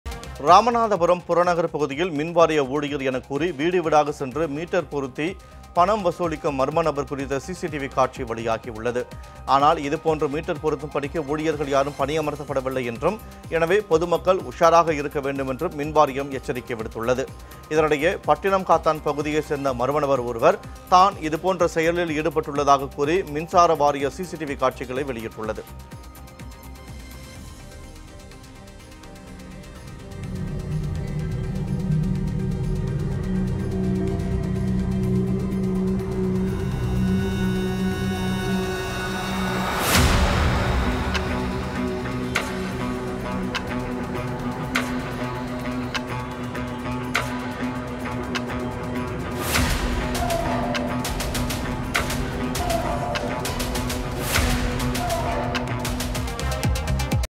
மின்வாரிய ஊழியர் எனக்கூறி வீடு வீடாக சென்று மீட்டர் பொருத்தி பணம் பறிப்பு | Ramanathapuram
மின்வாரிய ஊழியர் எனக்கூறி வீடு வீடாக சென்று மீட்டர் பொருத்தி பணம் பறிப்பு | Ramanathapuram | Jaya Plus